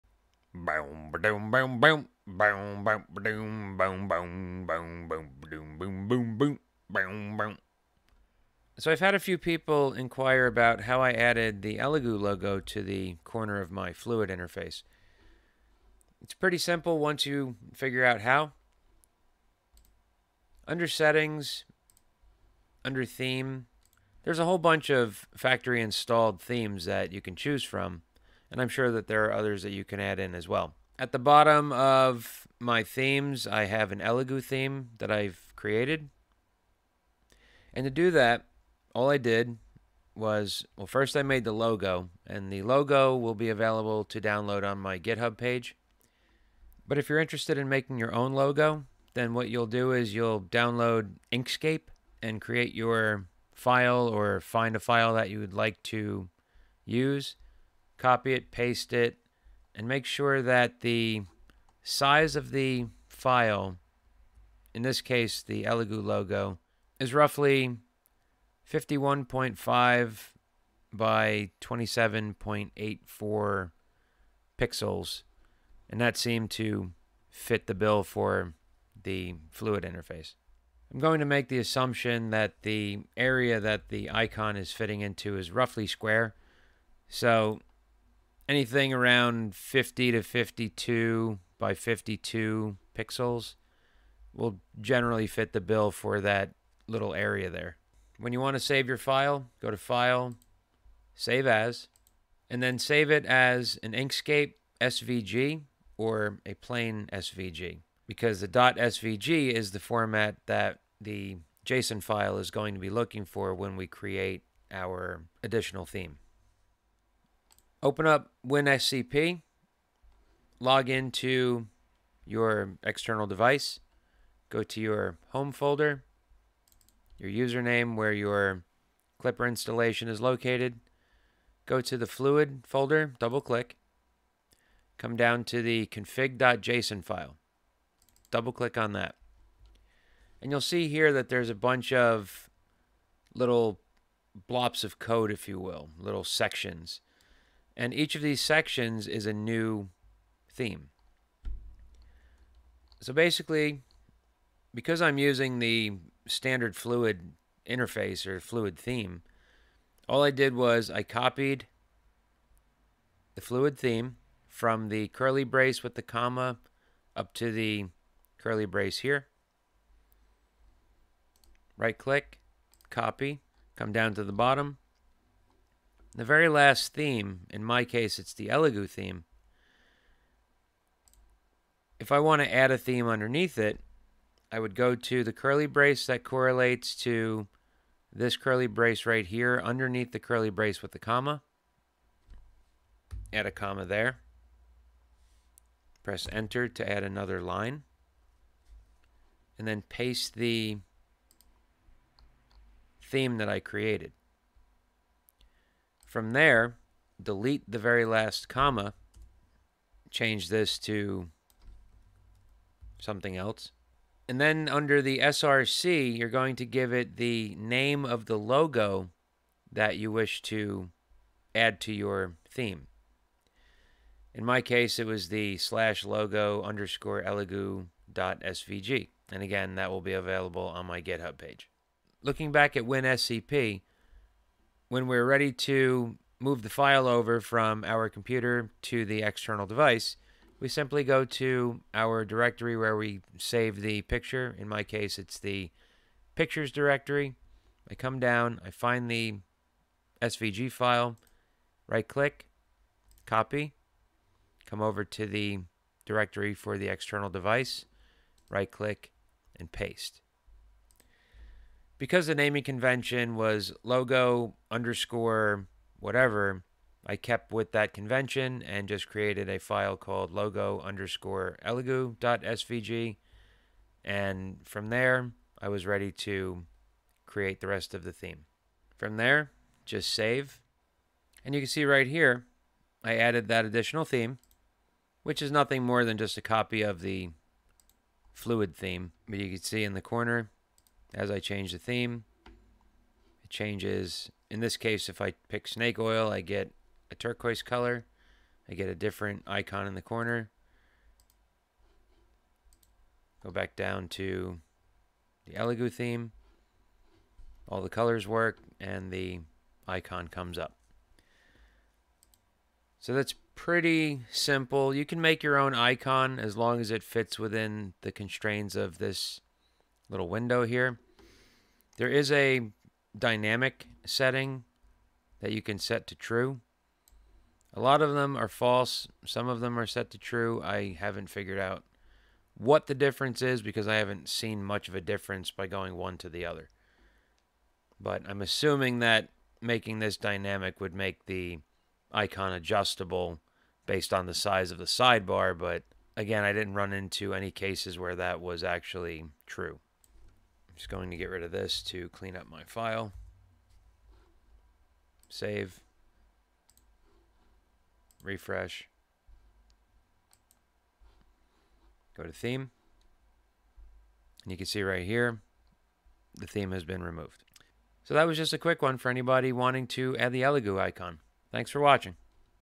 Adding a custom theme to Fluidd for the Elegoo Neptune (or any) 3D Printers
It's the little things that really matter sometimes. Here's a brief explanation on how to add a custom icon to your Fluidd interface